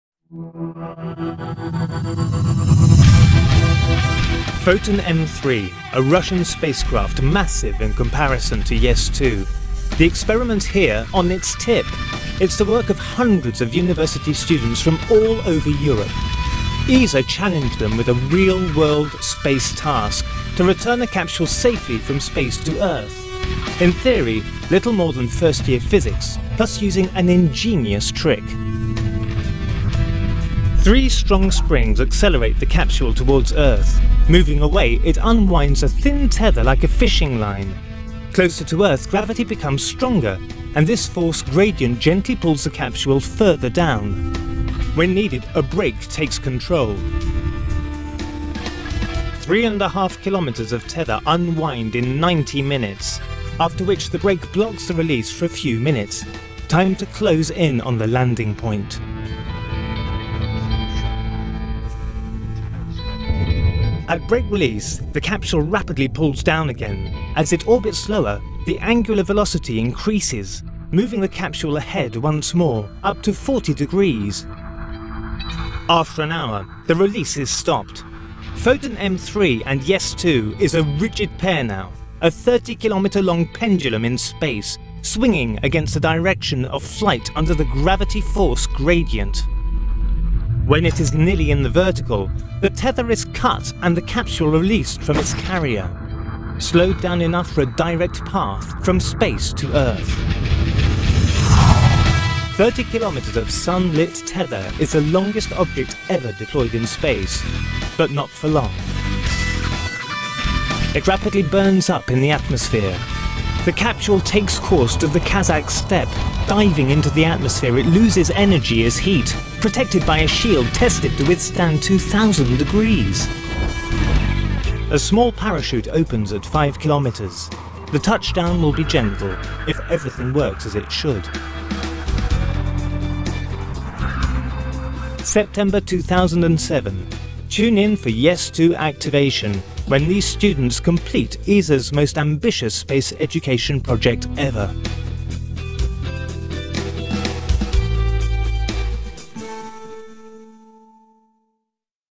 YES2 team claims a space tether world record
'On 25 September, students around the world watched with bated breath as their creation, the second Young Engineers Satellite (YES2) experiment, reached its dramatic conclusion.

A day before the Foton-M3 spacecraft returned to Earth, a small re-entry capsule, named Fotino, was to be released from the end of a 30 km tether, the longest such structure ever to be deployed in space. However, no signal was ever received from Fotino and its fate has been uncertain ever since.

First indications, based on real-time data processed by the YES2 flight computer and released by Russian mission controllers, suggested that the tether only unwound about 8.5 km before Fotino was cut free, but engineers wanted to know the full story of Fotino's final hours. Now, after weeks of careful analysis, the YES2 team has informed the ESA Mission Review Board of its findings.'

News release- 8th November 07. Source